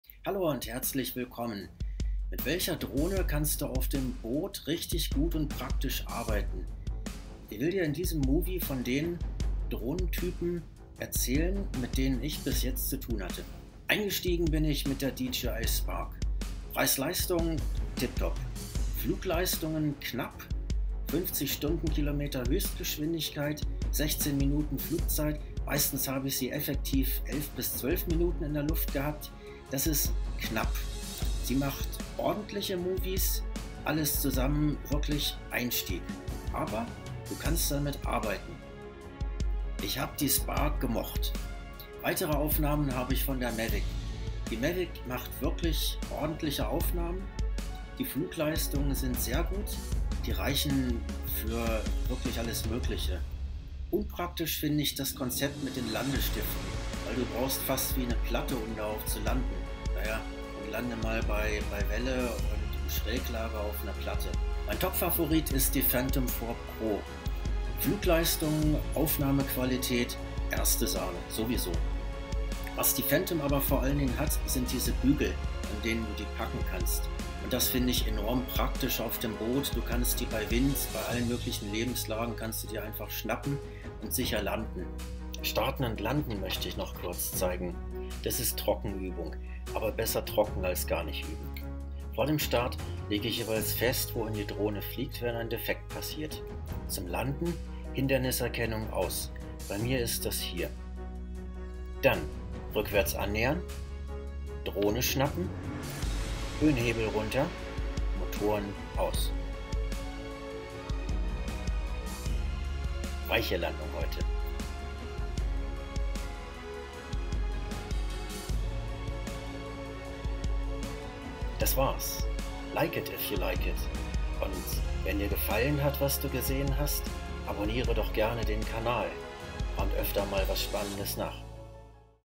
Welche Drohne für Boot Aufnahmen?
Welches ist die beste Drohne für Aufnahmen mit einem Boot? Meine Erfahrungen mit verschiedenen Drohnentypen findest du in diesem Movie.